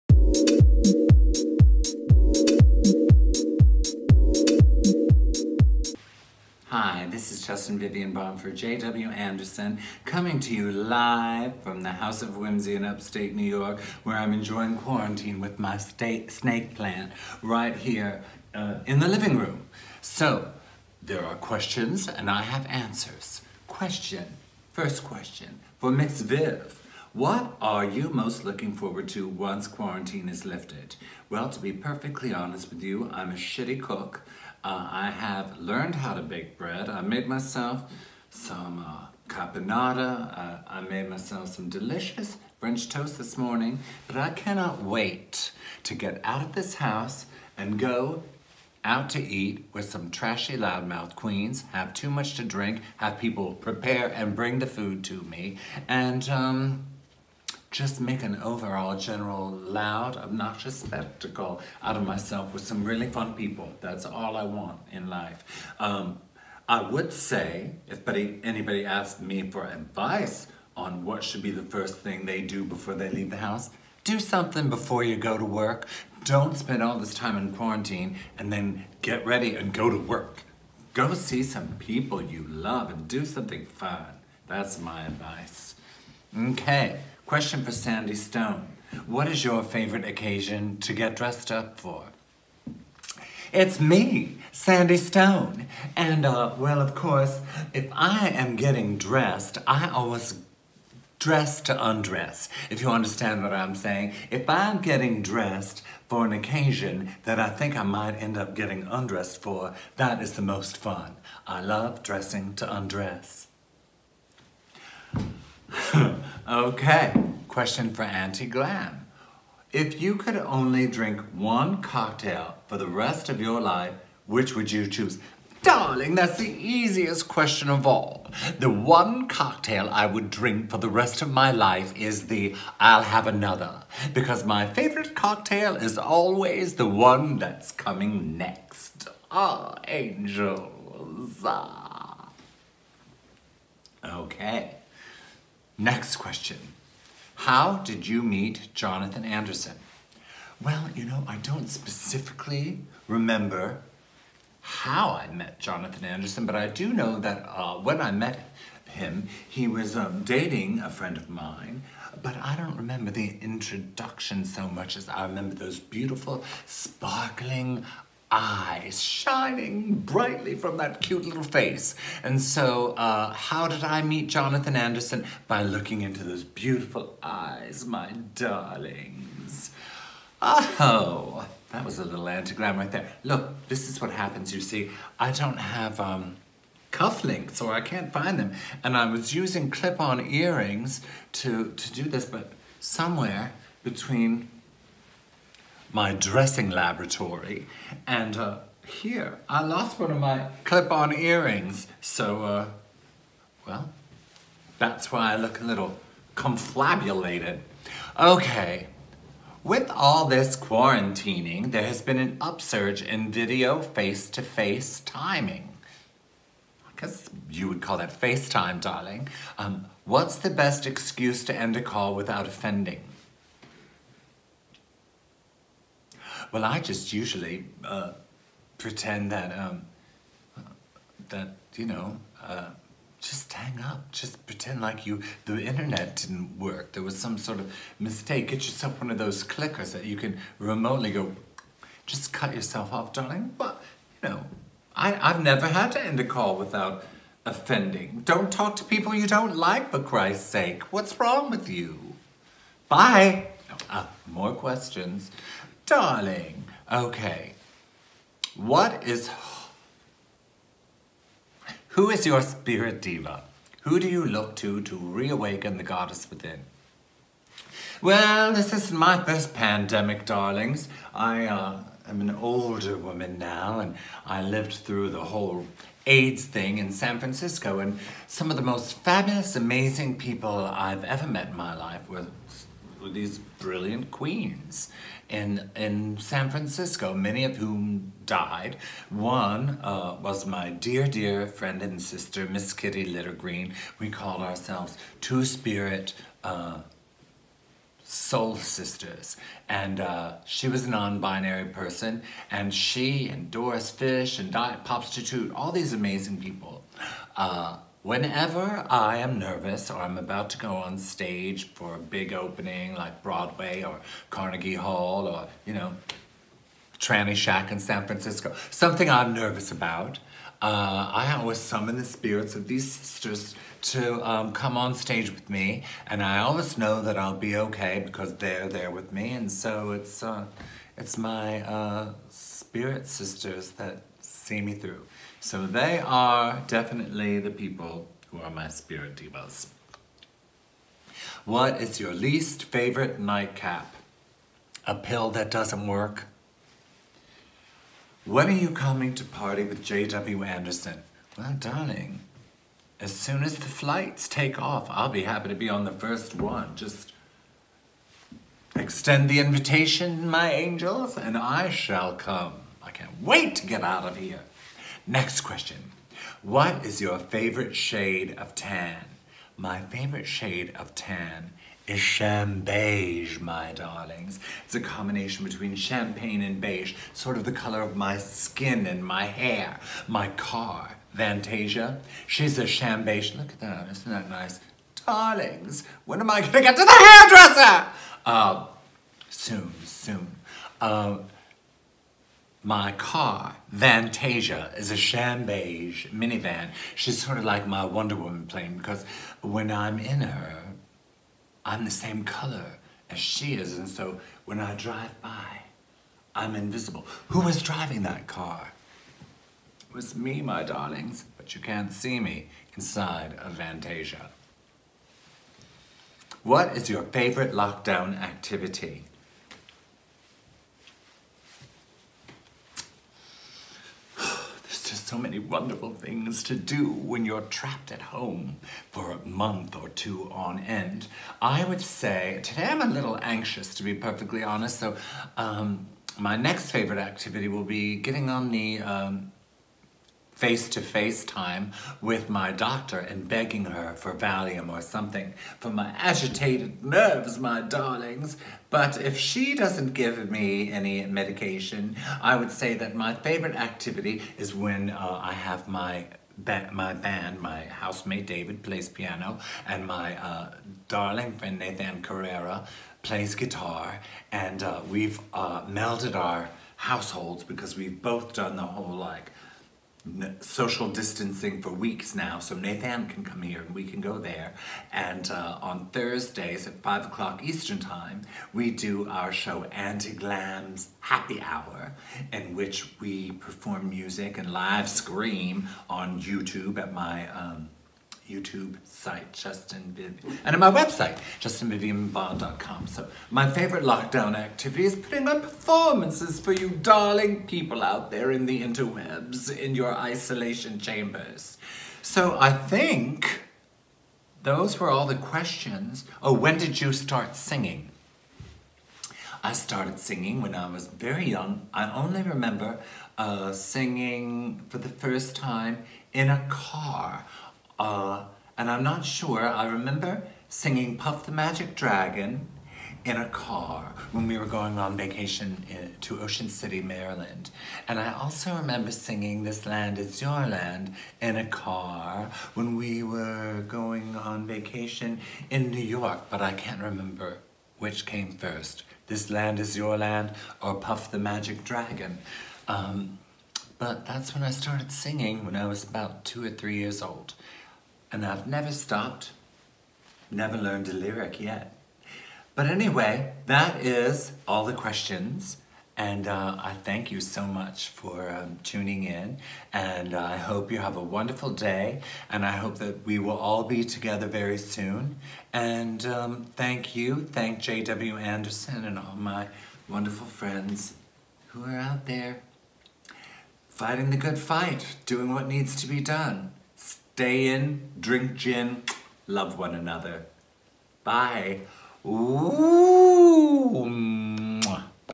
Mx Justin Vivian Bond Q&A | JW Anderson JWQ&A
Are you sitting down AtHome for this? Mx Justin Vivian Bond has answered your questions!